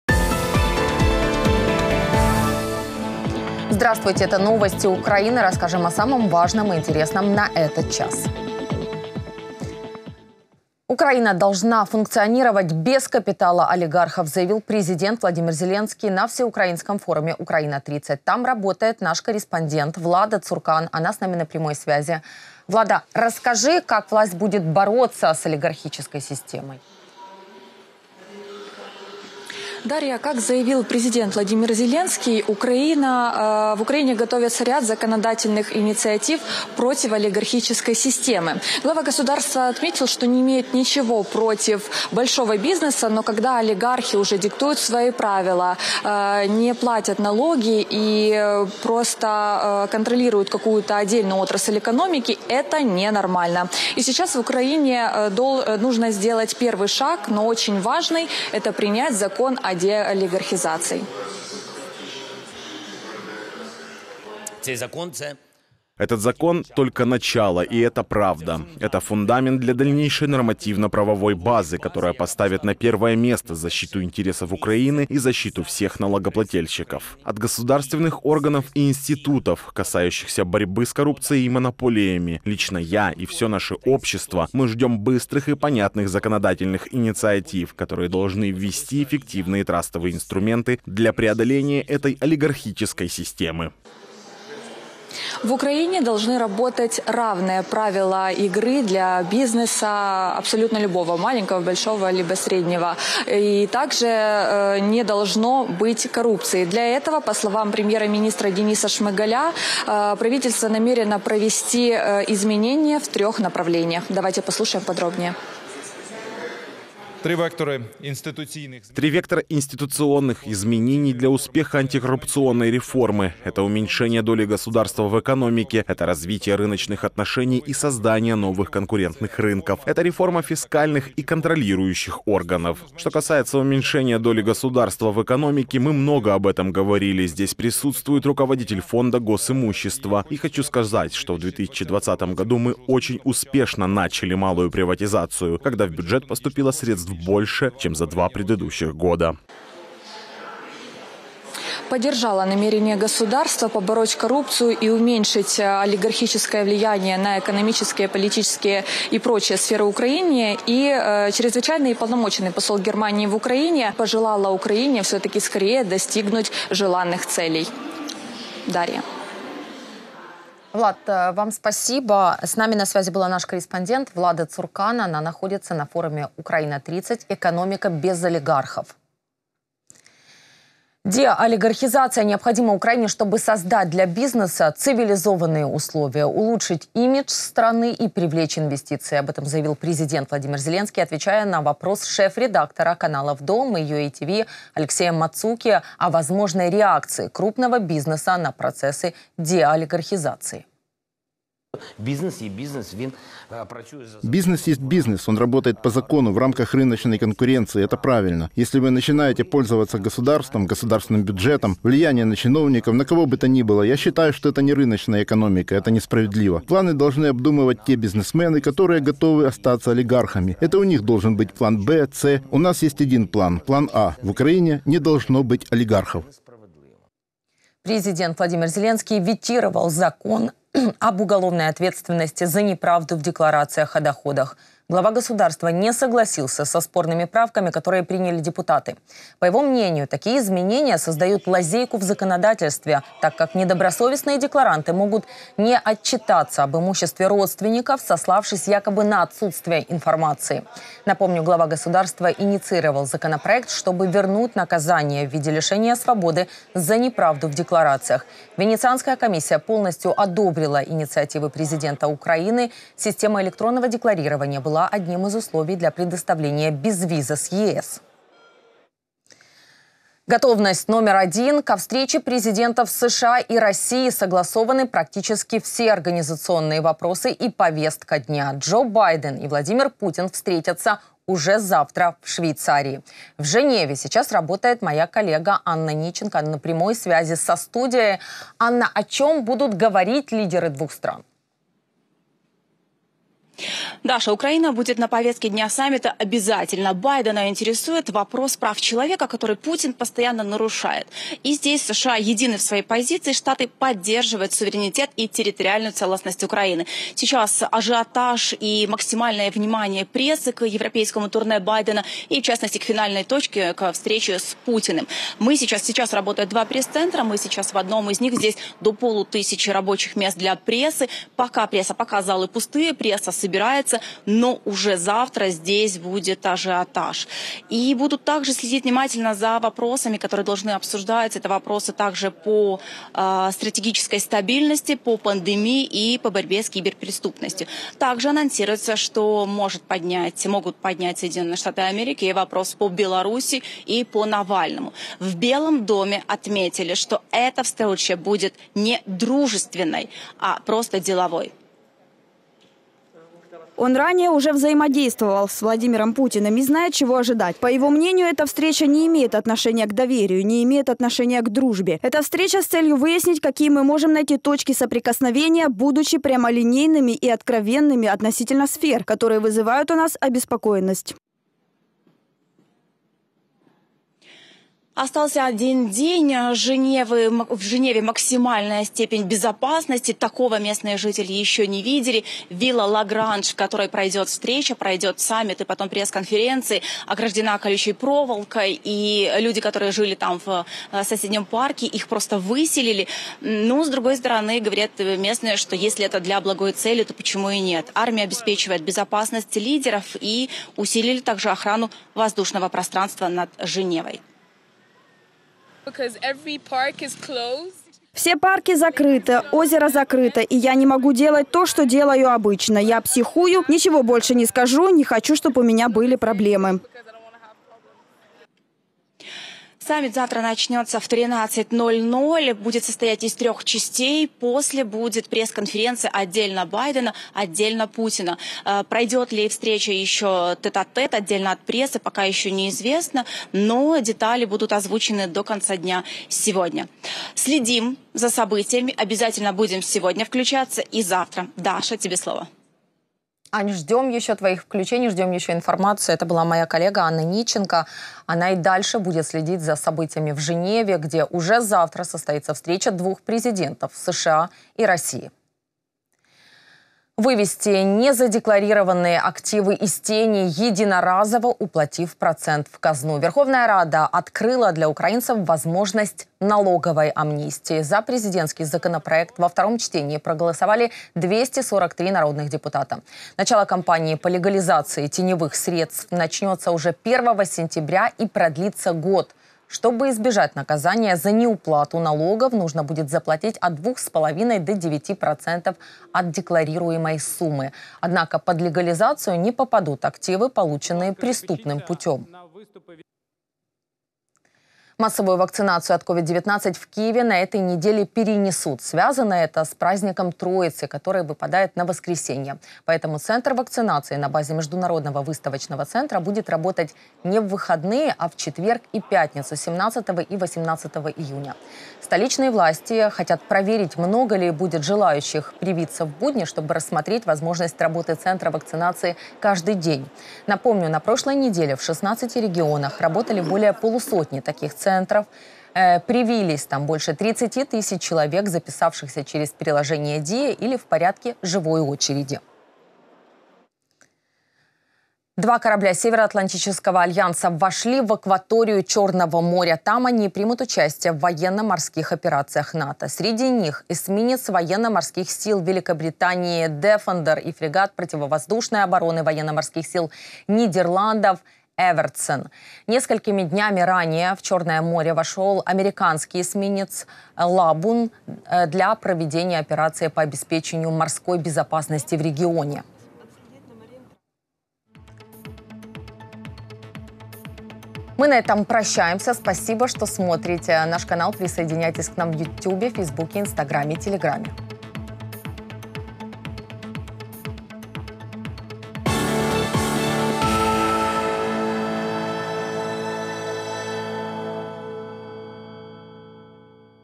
Экономика без олигархов. Главное с форума "Украина 30" | День 15.06.21
00:00 Смотрите в этом выпуске:
00:11 Украина должна функционировать без капитала олигархов, заявил президент Владимир Зеленский на всеукраинском форуме "Украина 30".
03:01 Деолигархизация необходима Украине, чтобы создать для бизнеса цивилизованные условия, улучшить имидж страны и привлечь инвестиции. Об этом заявил президент Владимир Зеленский.
03:47 Президент Украины ветировал закон об уголовной ответственности за неправду в декларациях о доходах.
04:30 Ко встрече президентов США и России согласованы практически все организационные вопросы и повестка дня. Джо Байден и Владимир Путин встретятся уже завтра в Швейцарии.
07:37 Верховная Рада открыла для украинцев возможность налоговой амнистии.
08:14 Центр вакцинации на базе Международного выставочного центра будет работать не в выходные, а в четверг и пятницу – 17 и 18 июня.
08:55 Два корабля Североатлантического альянса вошли в акваторию Чёрного моря. Там они примут участие в военно-морских операциях НАТО.